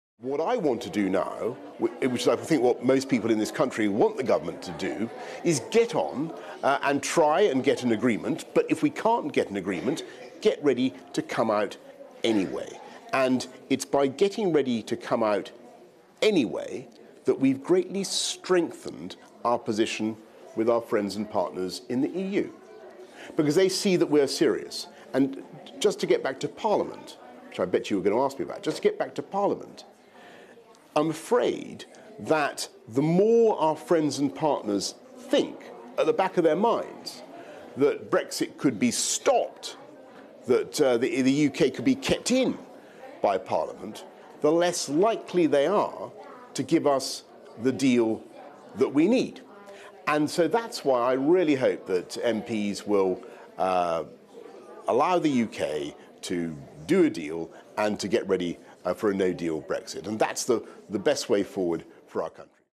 Boris Johnson says Remainers are jeopardising Brexit deal
Boris Johnson said the frantic efforts by MPs to block No Deal made the EU less likely to compromise on the Withdrawal Agreement. When deciding whether to give ground, European leaders would have 'at the back of their minds' the idea that Brexit could still be cancelled. The stark message, in a round of broadcast interviews, came after Tories confirmed they have been discussing with Speaker John Bercow how they could try to bind the PM's hands. Mr Bercow interrupted his family holiday in Turkey to issue an extraordinary tirade at Mr Johnson for announcing that Parliament will be prorogued .